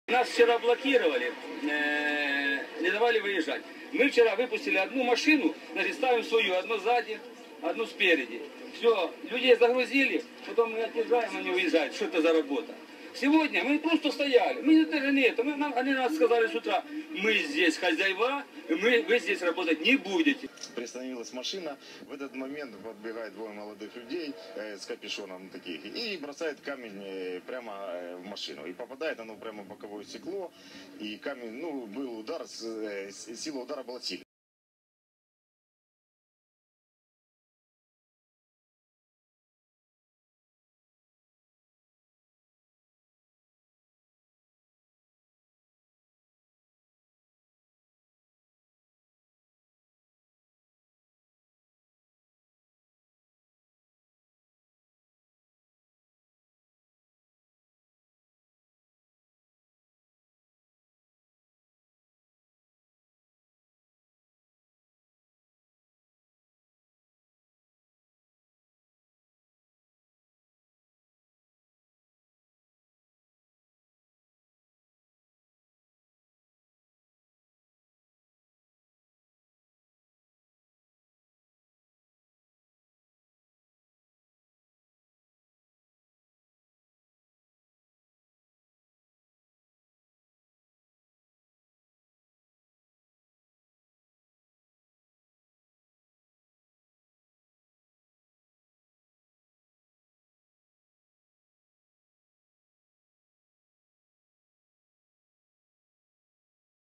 Оперативная съемка рэкетиров. Киев. Вокзал.
В Киеве на вокзале задержана "колхозная Коза Ностра". Оперативное видео, публикуется впервые.